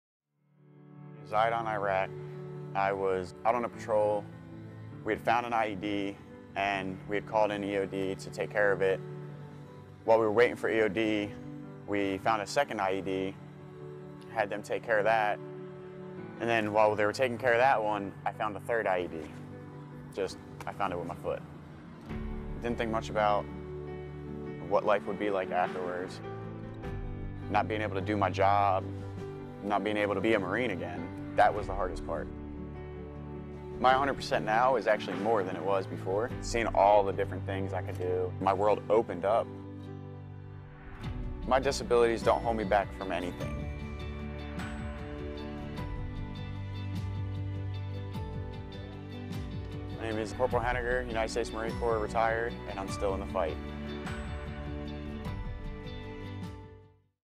Still in the Fight: Cpl. Ray Hennagir, 2015 Marine Corps Trials
The Marine Corps Trials is a chance for wounded, ill, and injured Marines, both past and present, to train as athletes, while increasing their strength so they can continue their military service or develop healthy habits for life outside the service. Retired Marine Cpl. Ray Hennagir, an Orlando, Florida native, tells us his story.